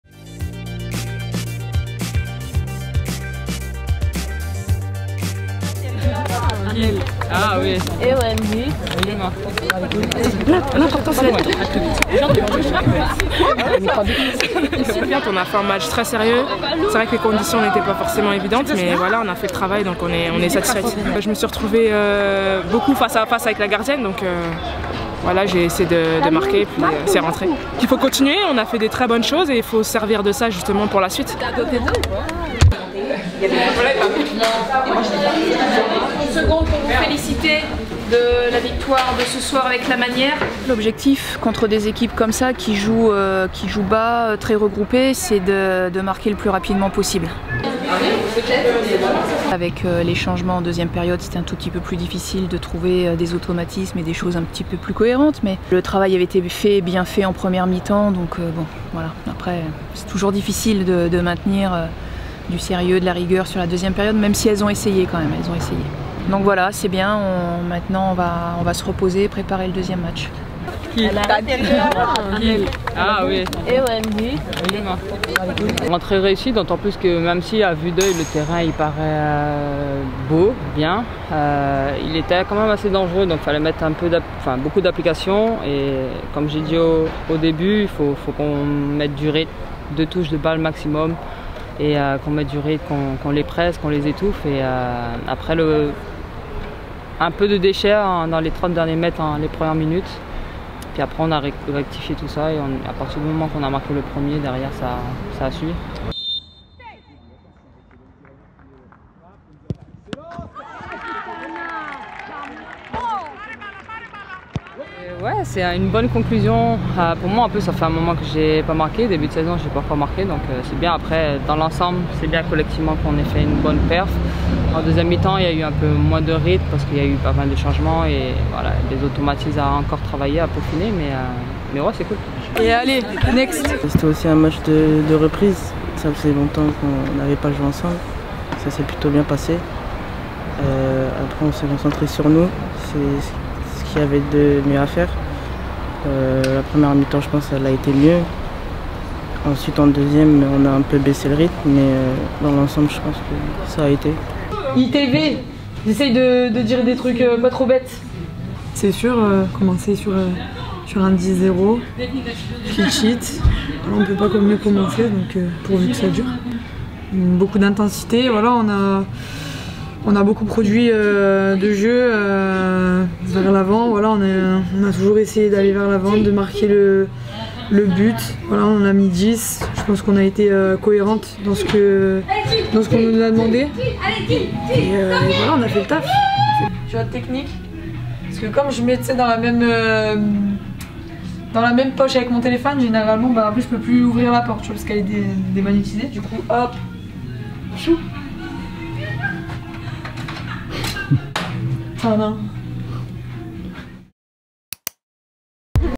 Grèce-France Féminines : 0-10 : premières réactions I FFF 2021
Pour leurs débuts dans les éliminatoires du Mondial 2023, les Bleues ont signé un festival offensif à Patras. Elles poursuivront mardi leur parcours avec un nouveau déplacement, en Slovénie (21h, sur W9).